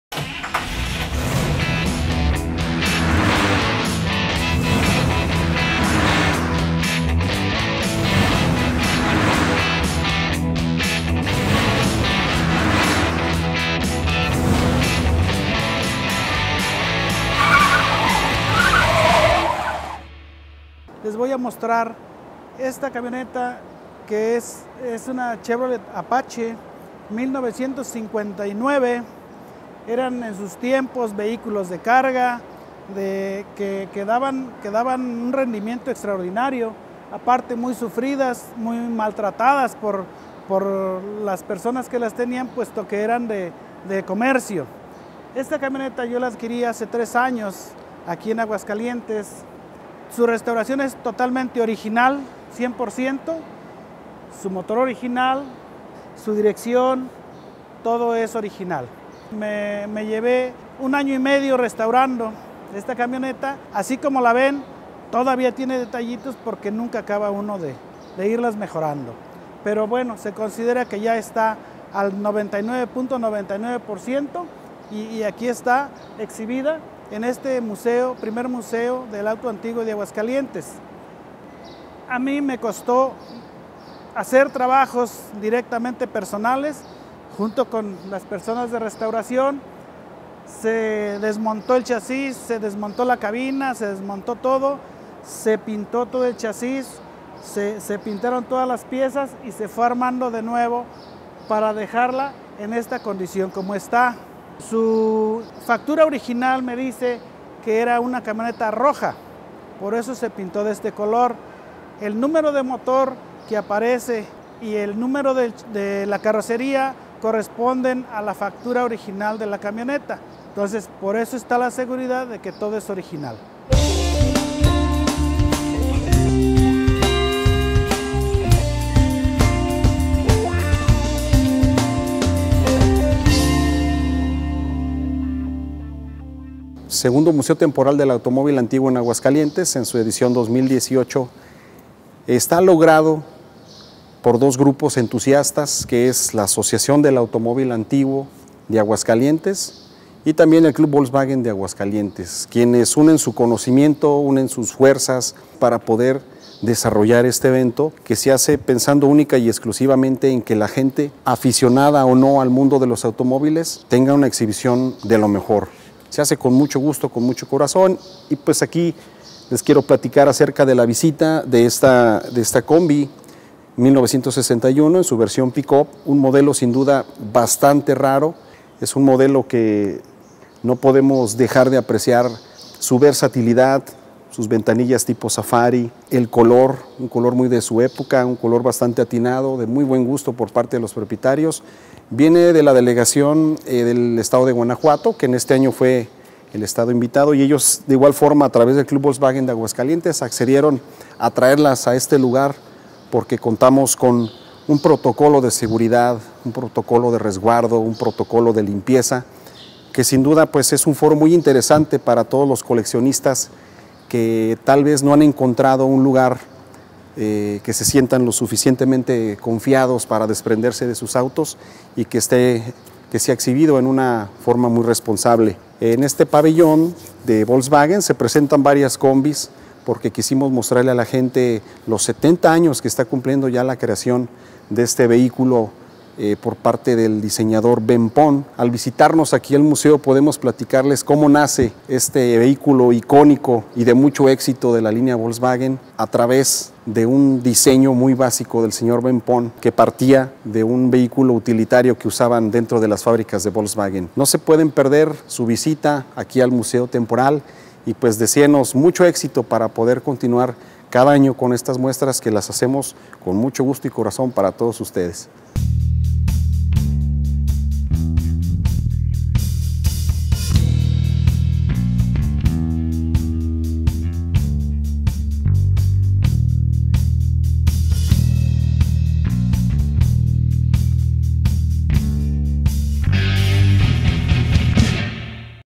Historias del volante-combi P05
Uno de los coches mas icónicos de todos los tiempos "La Combi"(en México), de Volkswagen,en esta capsula les mostramos algo de las especificaciones de esta preciosa camioneta y su diversidad de personalizaciones en colores y estilos; véanlos y comenten cual es su favorito.